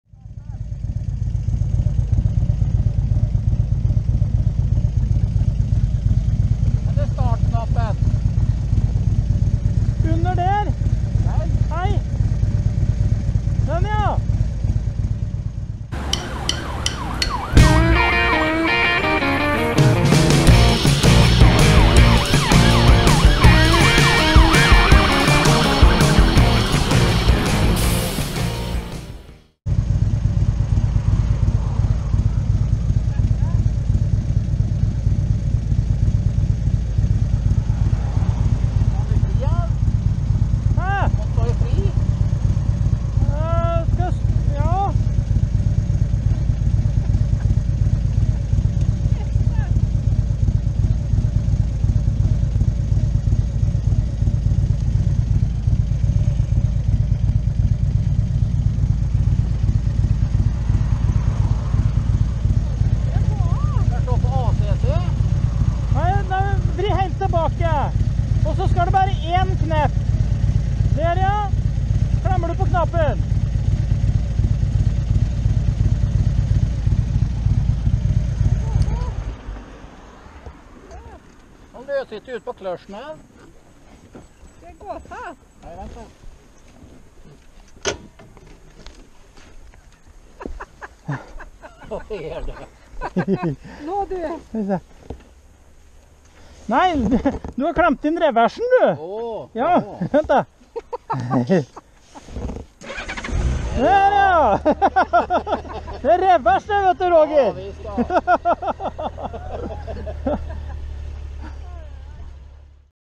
Roger finner reversen på Goldwingen Bumblebee.
Det kan by på utfordringer å kjøre hverandres motorsykler. Både Harley sin ElectraGlide og Goldwingen har mange knapper. Goldwingen har revers, og den knappen sitter kanskje litt for nærme startknappen. Det fikk vi erfare på denne superfine turen til Nedergårdens Vilt & Natur, Nedergården, Sysslebäck. Dette er med og skaper gode minner og god latter.